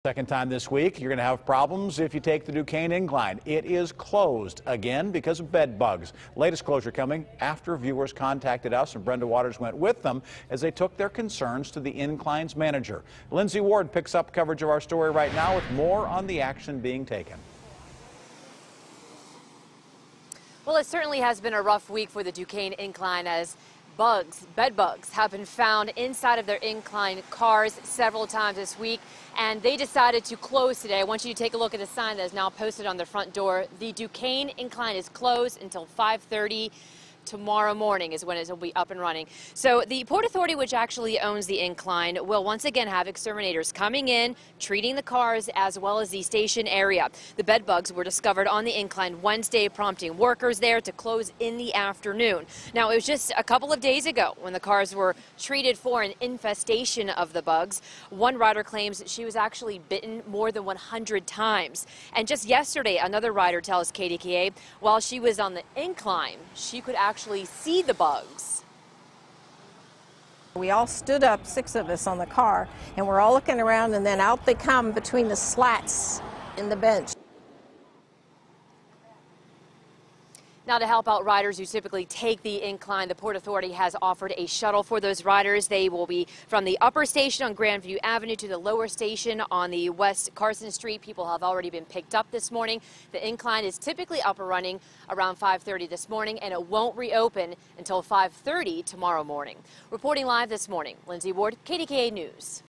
Duquesne Incline Closed For Bed Bug Treatments
The Duquesne Incline is closed once again due to concerns about bed bugs; KDKA's Lindsay Ward reports.